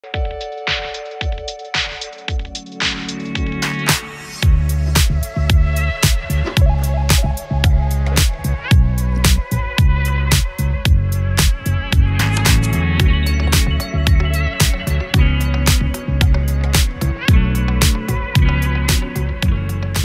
Small Inflatable Dinghy assembly! 9ft Green 2 person boat!
Inflatable motor boat CRB-BT270DB is a two-person motor boat with two sliding seat and a book-type flooring (rigid folding floor made of wood blocks connected to each other with PVC strips). This is one of the most light-weight and compact CRB motor boats with a stationary transom.